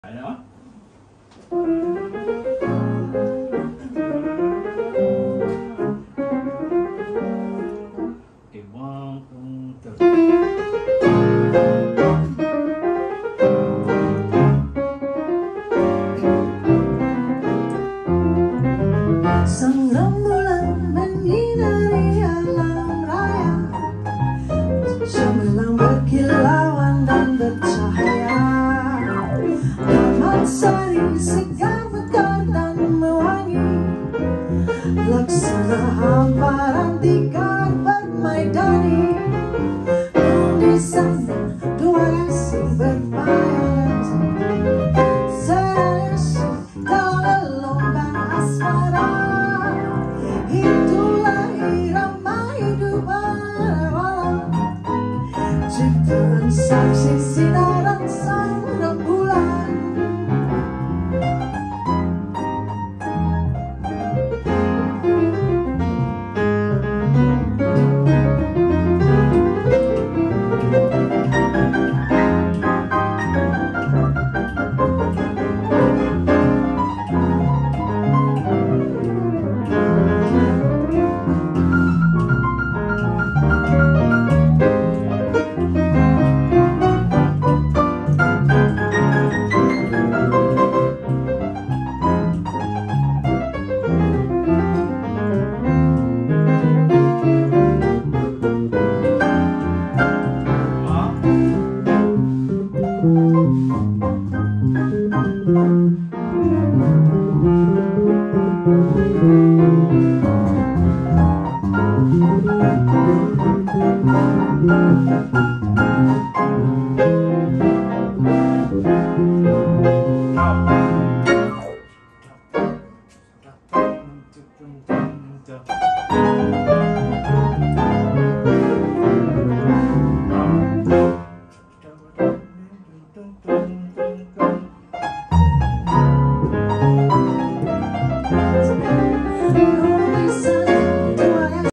Ida Mariana and Kebaya Jazz Trio @ Jaotim on 16 Sept@830pm
16 Sept 830pm - Trio of DM Amar n Sudin with Ida Mariana at Jaotim playing Nusantara Jazz